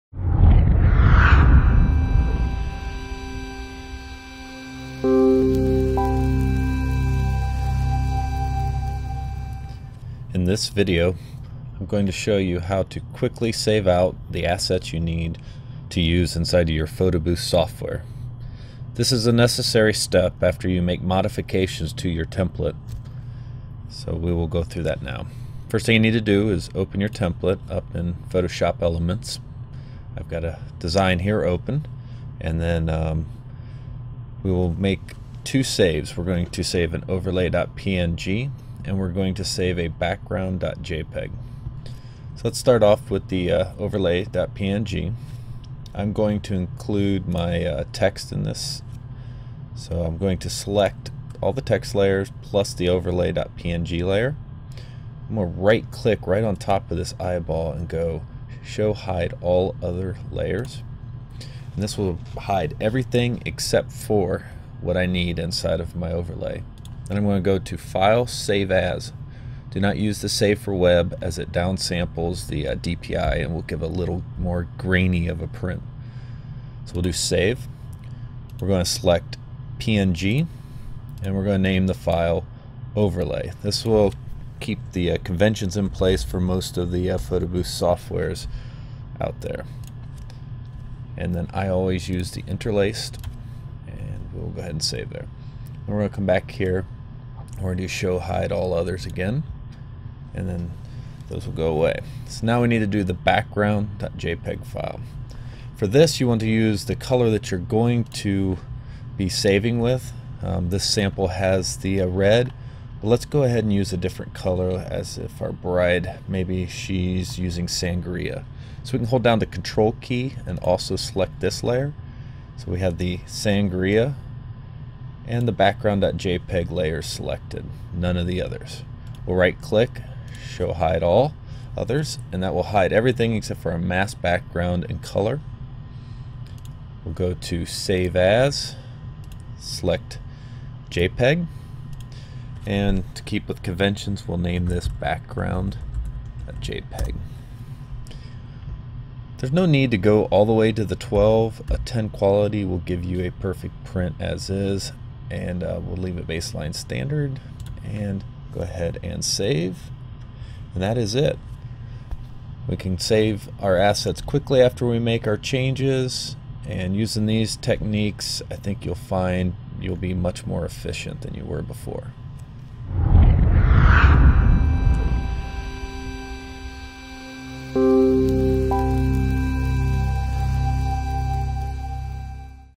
Photoshop Elements Saving Template Assets
- In this video I show you how to save the template assets in Photoshop Elements for your photo booth software. This is necessary whenever you make modifications to the layout.

I will...
1 - Show you how to hide all but the overlay portion of the template design in Photoshop Elements.
2 - Show you how to properly save the overlay.png file for use in your photo booth software.
3 - Show you how to hide all but the background portion of the template design in Photoshop Elements.
4 - Show you how to properly save the background.jpg file for use in your photo booth software.